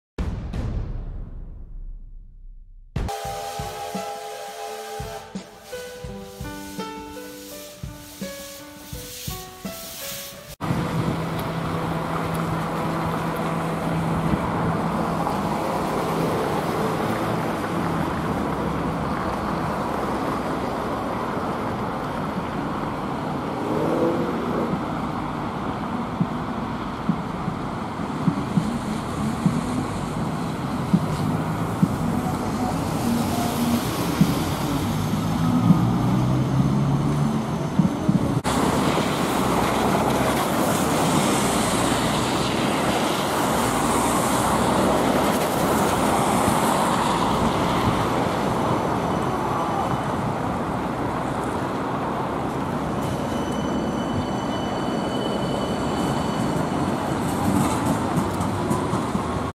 Трамваи "Витязь-М" (71-931М) №8918 и №8915
Трамваи "Витязь-М" (71-931М) №8918 и №8915 (Приписаны и обслуживаются ГУП "ГорЭлектроТранс",трамвайный парк №8)
Маршрут №60 -
"Завод "Северная Верфь" - ЛЭМЗ" и "ЛЭМЗ - завод "Северная Верфь"".

Ул.Маршала Казакова/ул.Десантников,Санкт-Петербург,Россия.
14.III.2021